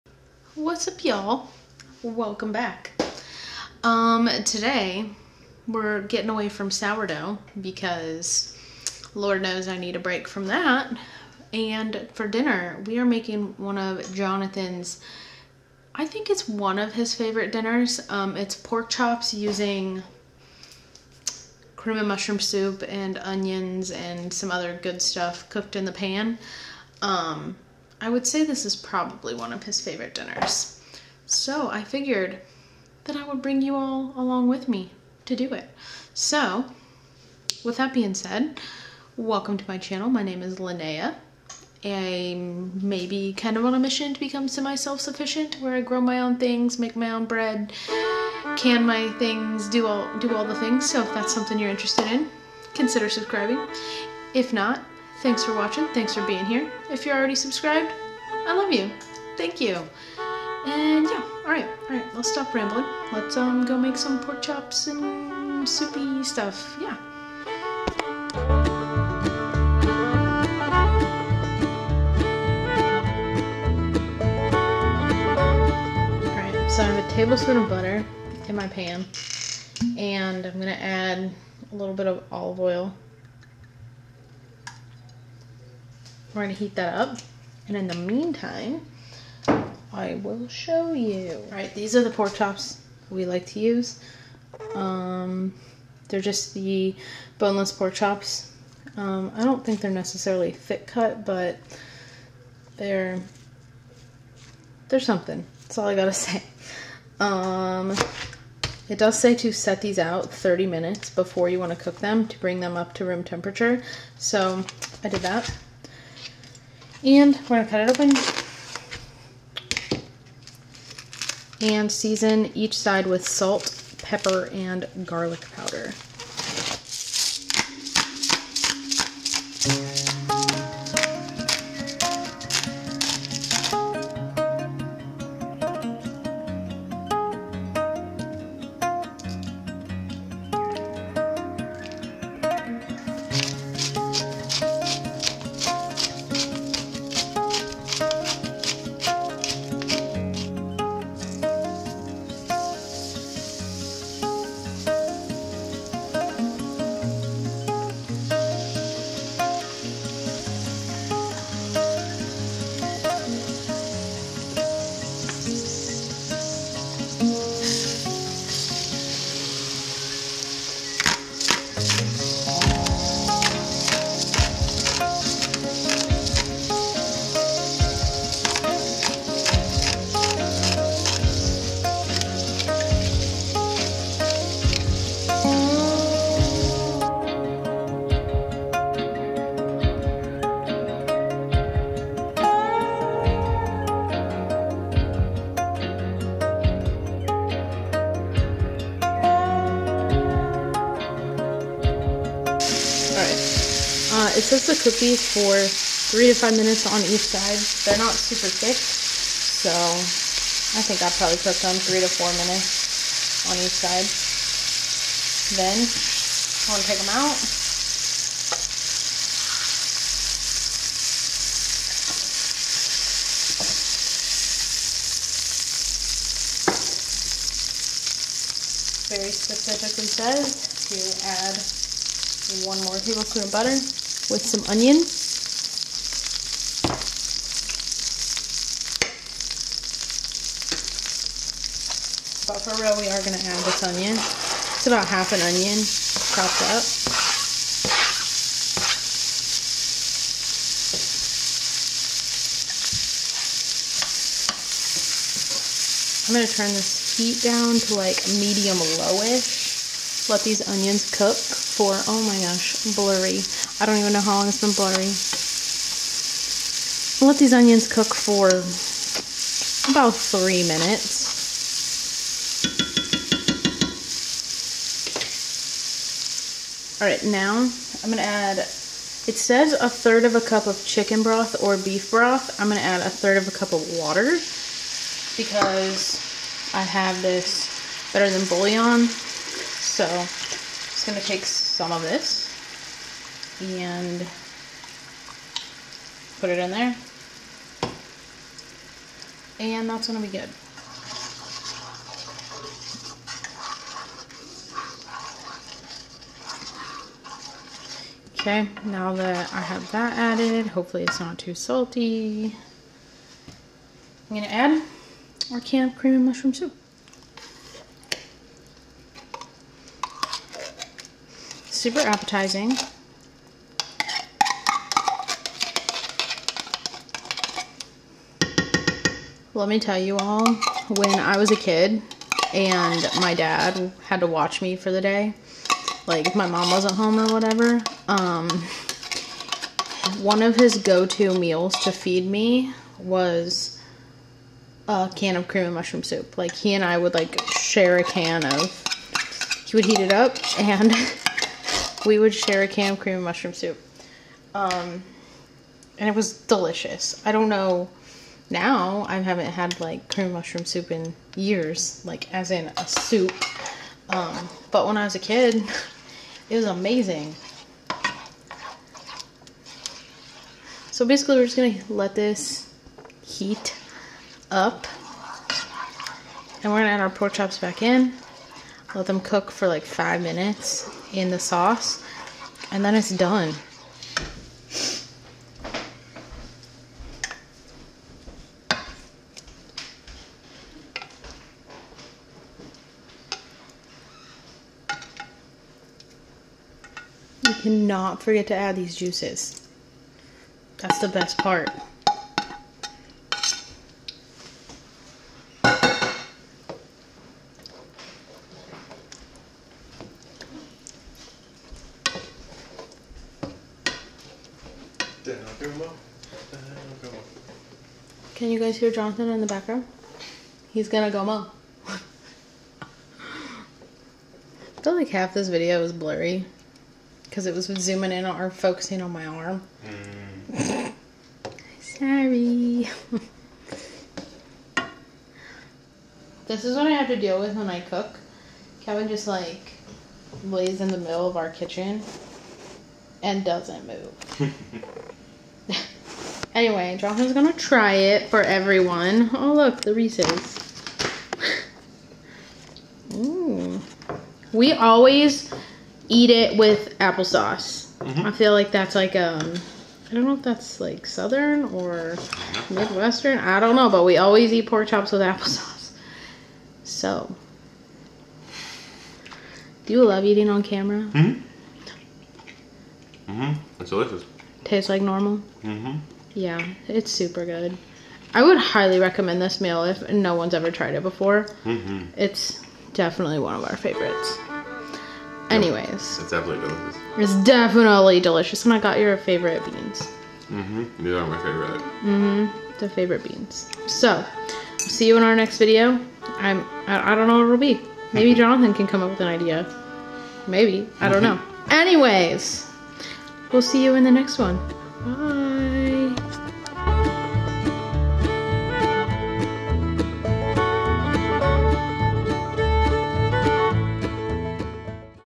30 Minute Meal Recipe | Cream of Mushroom Soup Pork Chops | Easy Recipe
In this video, I show you how to make one of my absolute favorite meals. It only takes about 30 minutes, so if you're in a time crunch or have a busy schedule, this recipe is for you! I show you step by step how to make this simple 30 min meal. This meal is also husband approved and picky eater approved!